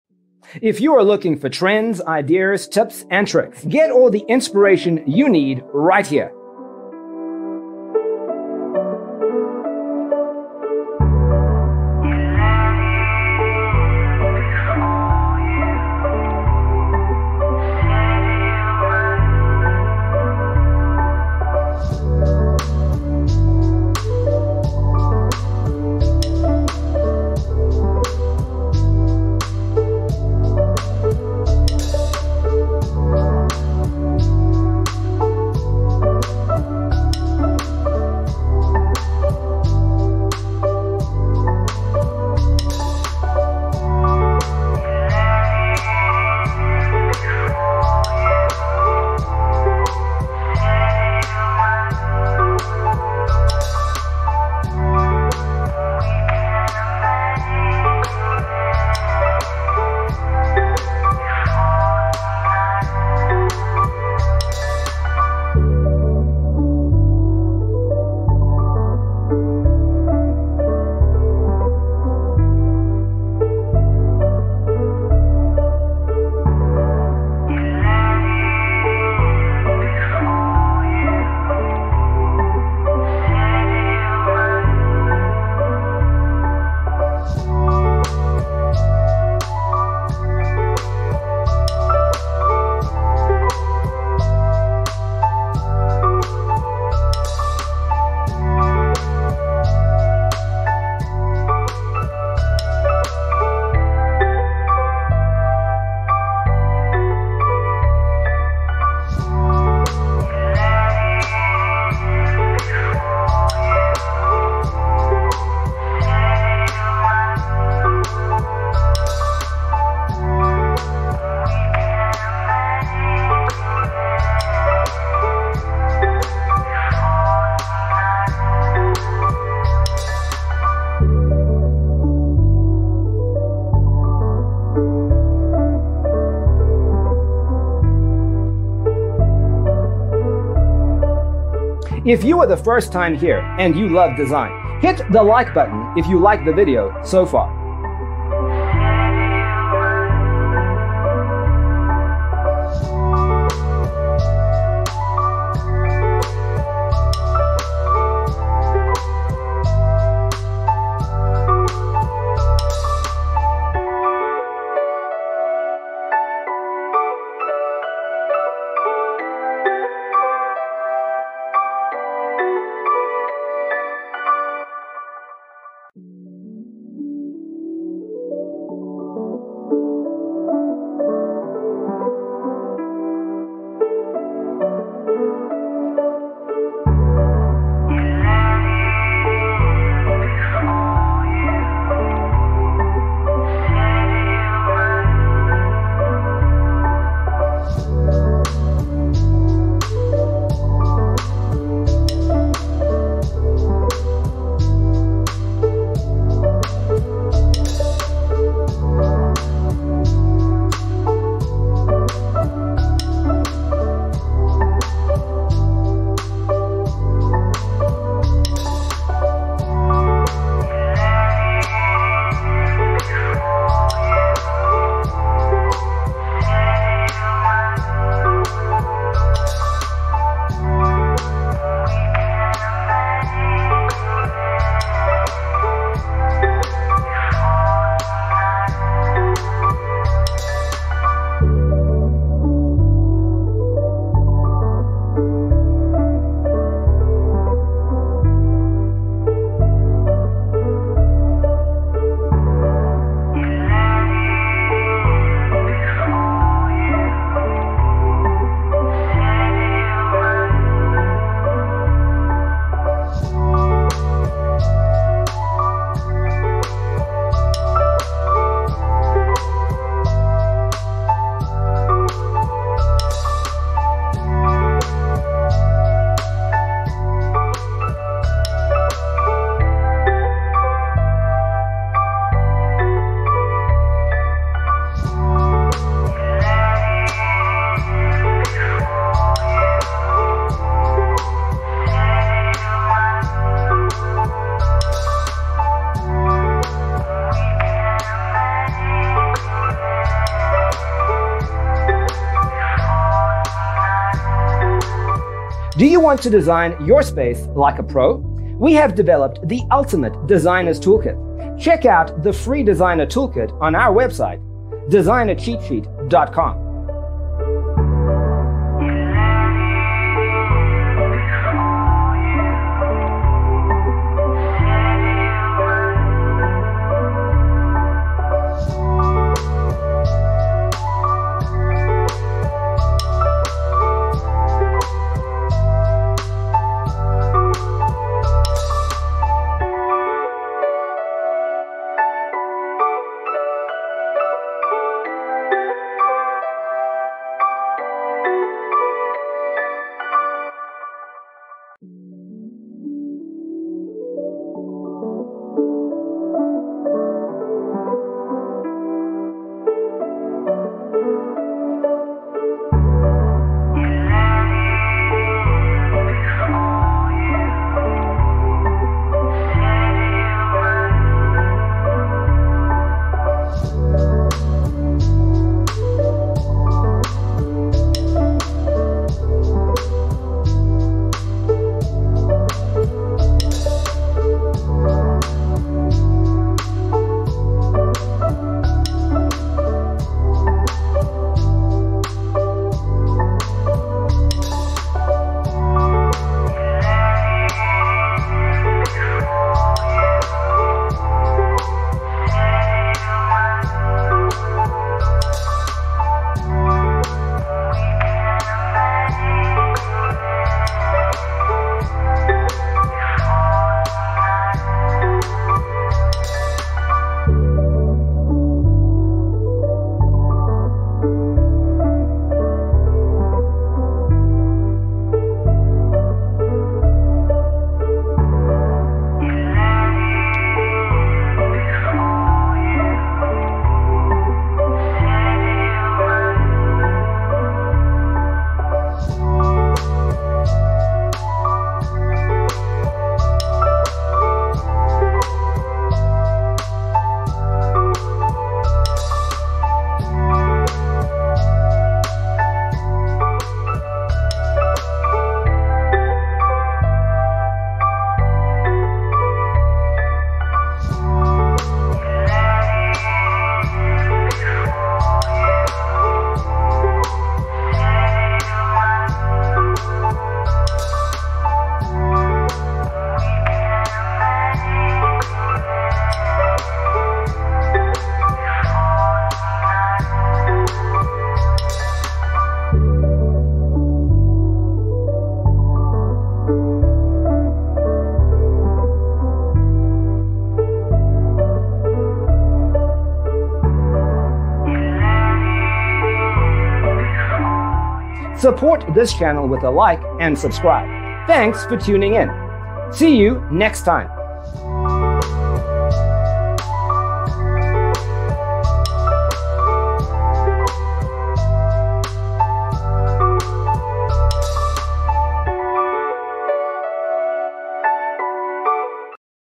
75 Craftsman Clapboard Exterior Home Design Ideas You'll Love 🌈
Browse craftsman exterior home design photos. Discover decor ideas and architectural inspiration to enhance your homes craftsman exterior and facade as you build or remodel.

0:00:00 - Intro
0:00:12 - Craftsman Clapboard Exterior Home Design Ideas You'll Love: Inspirations 1 to 25
0:03:03 - Craftsman Clapboard Exterior Home Design Ideas You'll Love: Inspirations 26 to 50
0:06:07 - Craftsman Clapboard Exterior Home Design Ideas You'll Love: Inspirations 51 to 75

Hello there!🌈